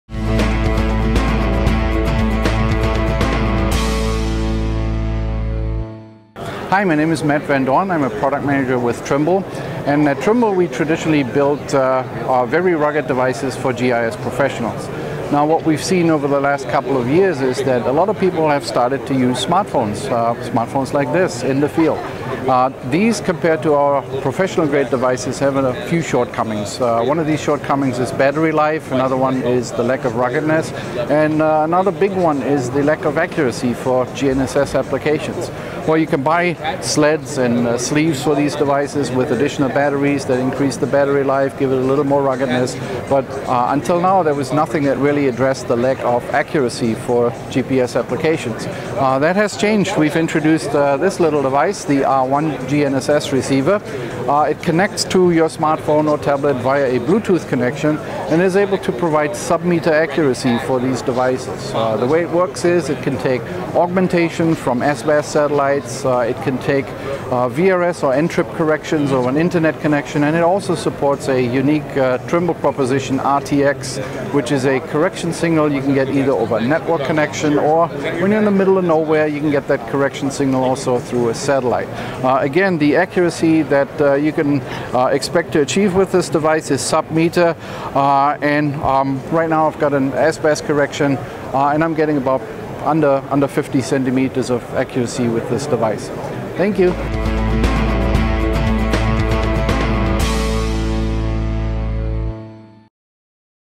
Esri UC: Trimble R1 GNSS Receiver Enables Data Collection with Smart Devices
Matt van Doorn of Trimble Navigation features the Trimble R1 GNSS receiver at the 2015 Esri User Conference, held July 20–24 in San Diego. The R1 is a rugged, pocket-sized GNSS receiver that provides sub-meter precision to users of any Bluetooth connected mobile device, including iOS, Android and Windows mobile smartphones, tablets, or more traditional integrated data collection tools such as a Trimble handheld computer. The R1 GNSS receiver supports multiple GNSS constellations, including GPS, GLONASS, Galileo, QZSS and BeiDou.